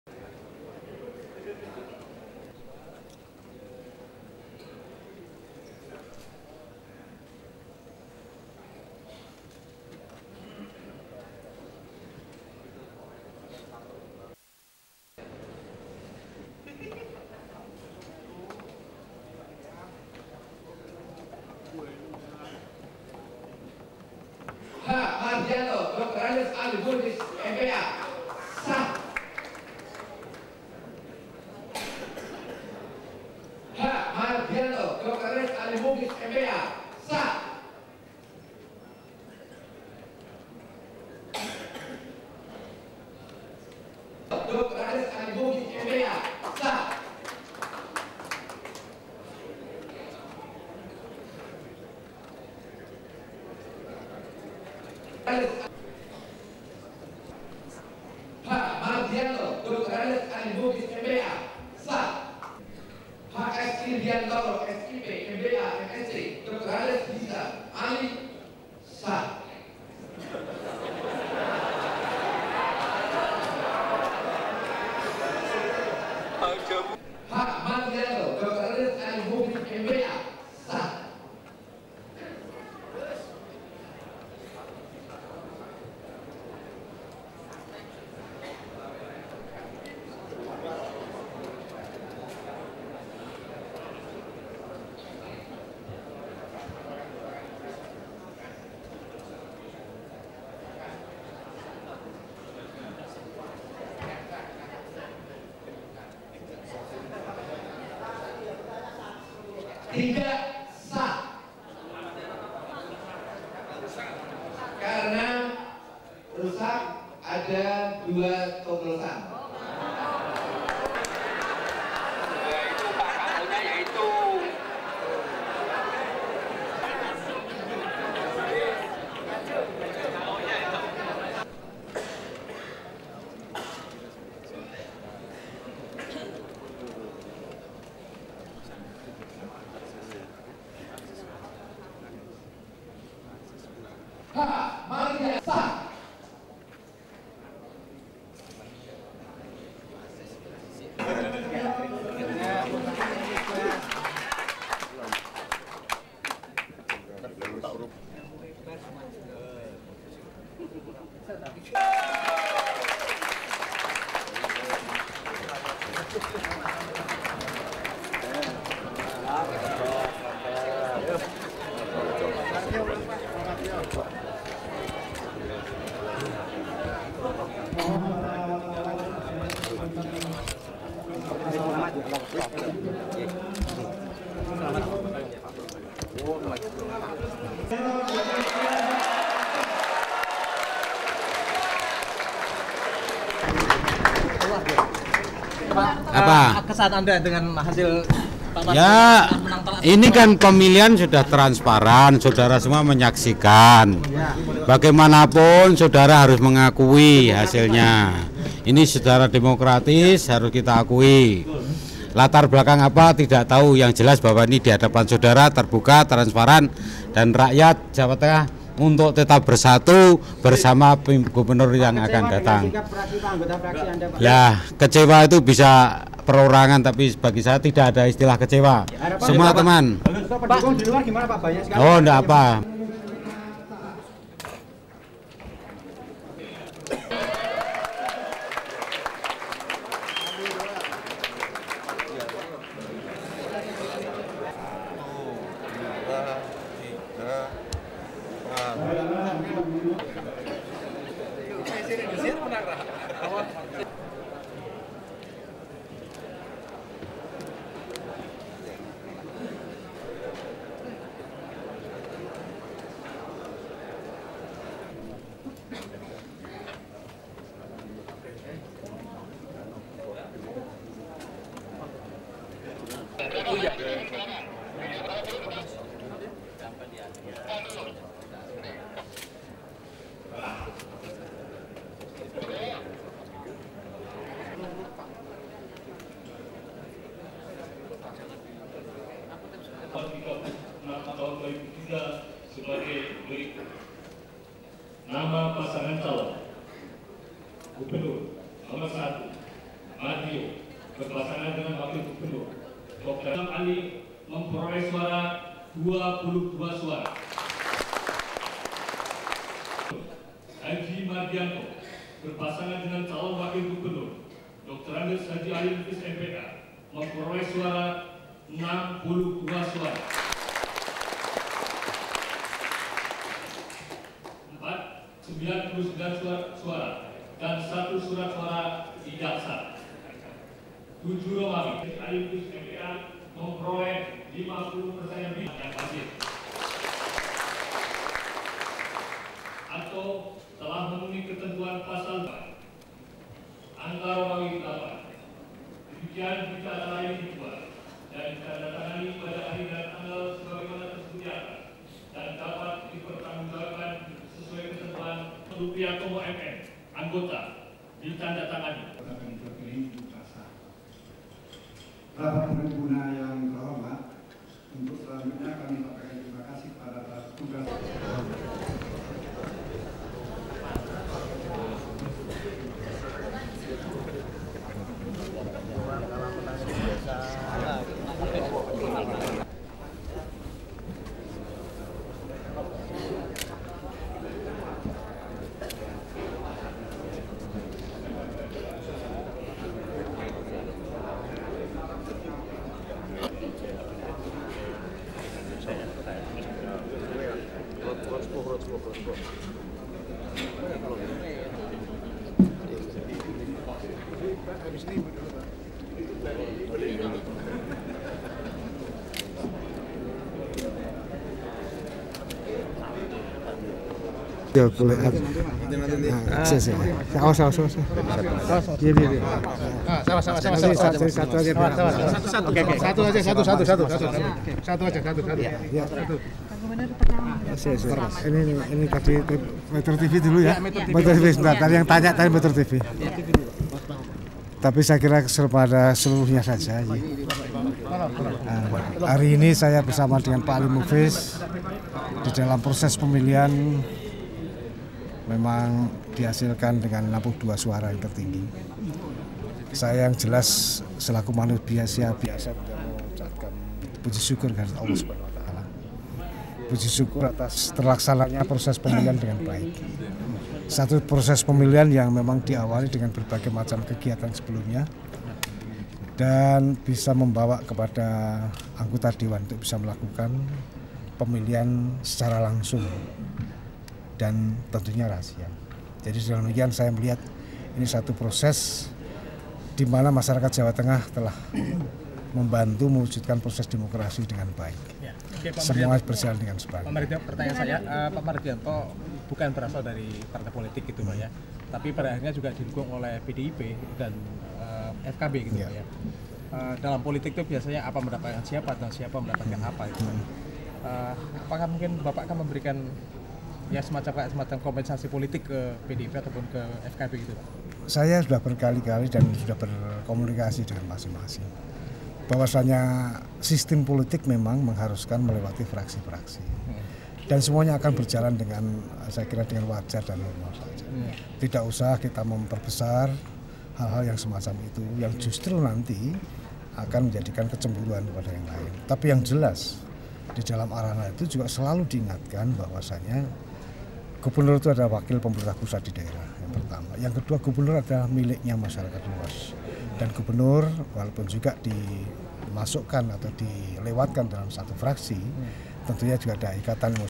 Pengumuman Hasil Pemilihan Gubernur Semarang Jateng Dok. 2003
MetroTV,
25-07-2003 Mardijo Di DPRD Jateng Tentang Pembacaan Hasil Pemilihan Gubernur Jateng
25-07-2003 Suasana Pemilihan Gubernur Semarang Jateng